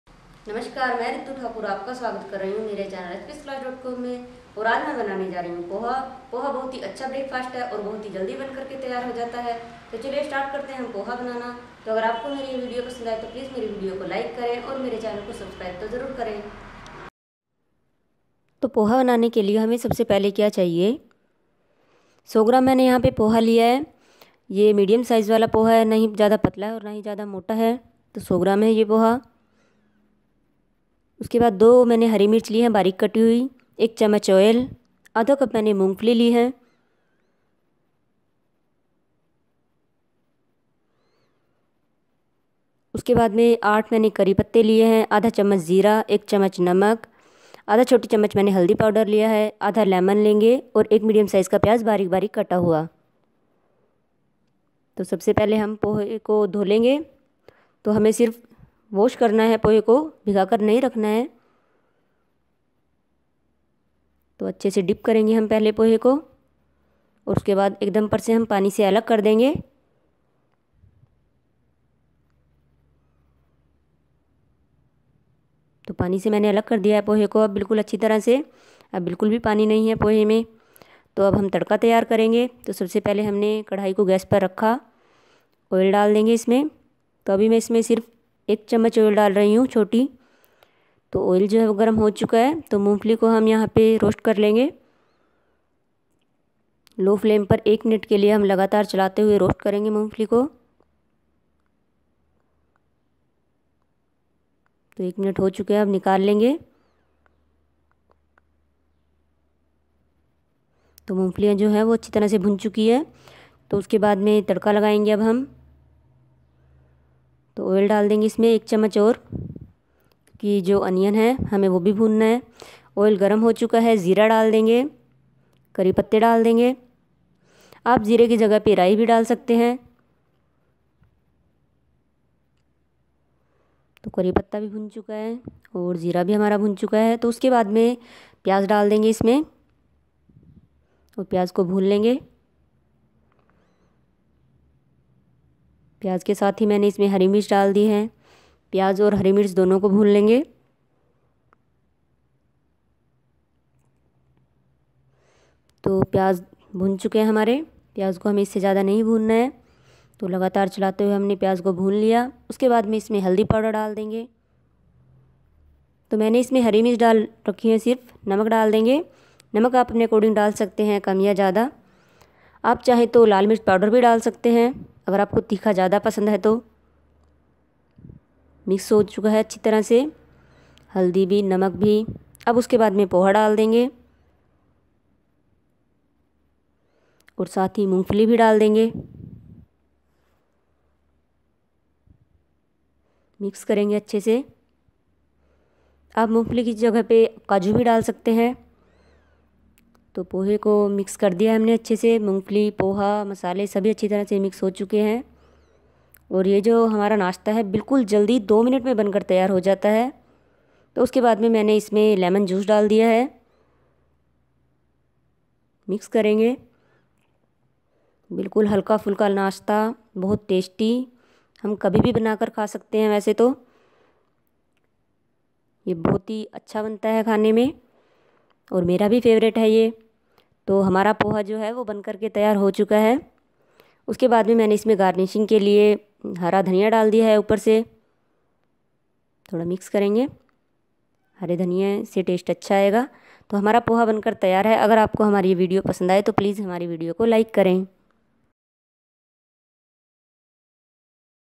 2 मिनट में पोहा बना कर खाए | Poha Banane ka Tarika
2  मिनट में पोहा कर खाए | Poha Banane ka Tarika : Hello friends today we are going to share a very tasty breakfast that is Poha Recipe.
This is very famous recipe.It is easy to make.